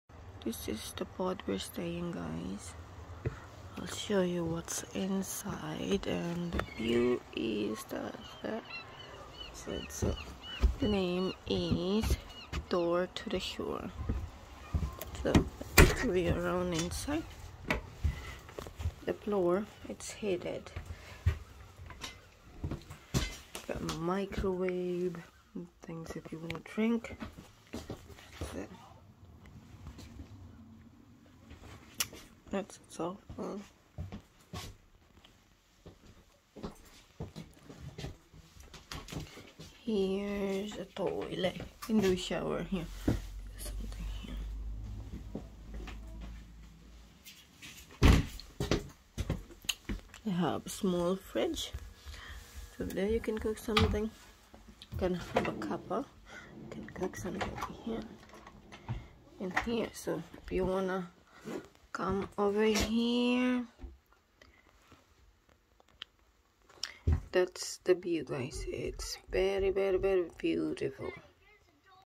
What's on the POD?
Hi guys.
I just want to show you the POD where we stay in for 2 nights.
It's a beautiful place to visit and the views are stunning.
The place is really quiet.
It's located in Sandhaven just 5 minutes drive to Fraserburgh Aberdeenshire or 10 minutes drive by Bus.
On this place there's a Chinese take away maybe i minutes walk and there's a shop name Premier about 8 minutes walk too.
We enjoyed staying on this place and we're so lucky the weather is great too.